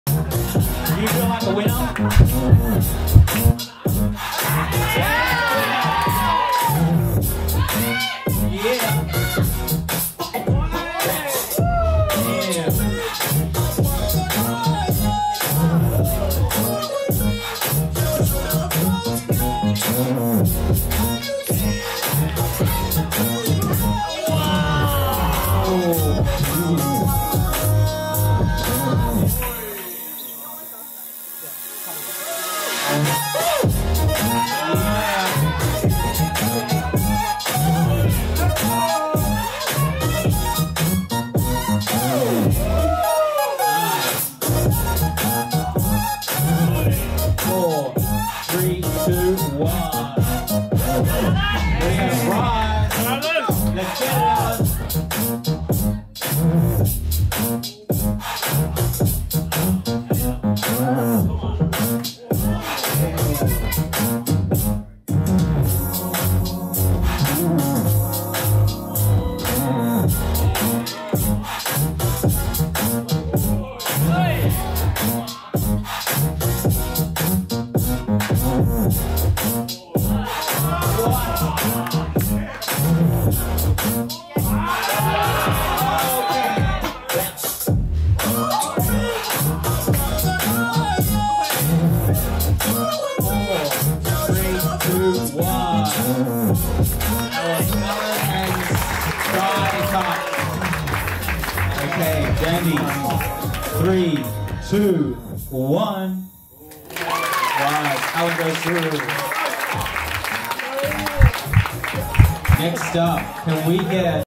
Shytut (SG) vs Rize | Popping Best 16 | DTB vol. 3
Filmed & Edited by Mighty Madman Media

Artwork by illustreet

Organised by Johann & AuX Poppers

Winner: Rize

DTB vol. 3 22nd September 2018 @ Club 80 Proof